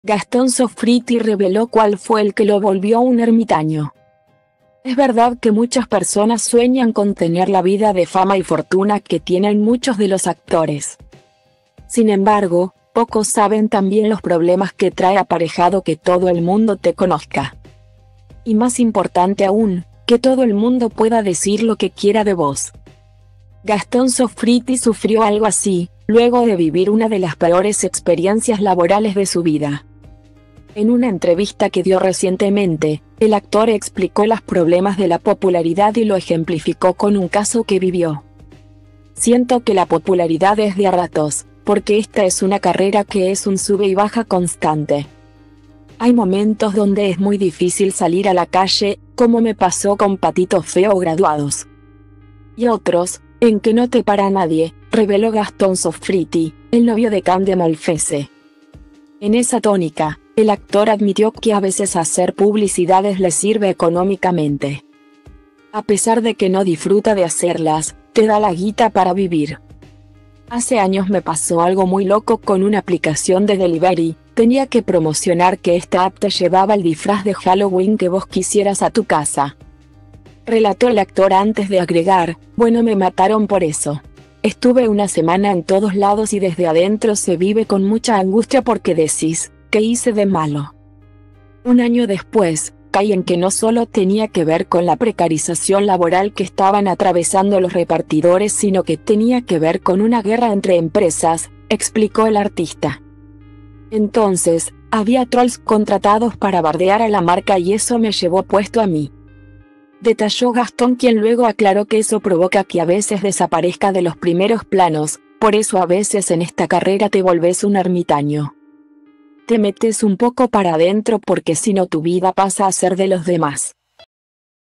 Gastón Soffritti reveló cuál fue el que lo volvió un ermitaño
Es verdad que muchas personas sueñan con tener la vida de fama y fortuna que tienen muchos de los actores,sin embargo, pocos saben también los problemas que trae aparejado que todo el mundo te conozca,y más importante aún, que todo el mundo pueda decir lo que quiera de vos.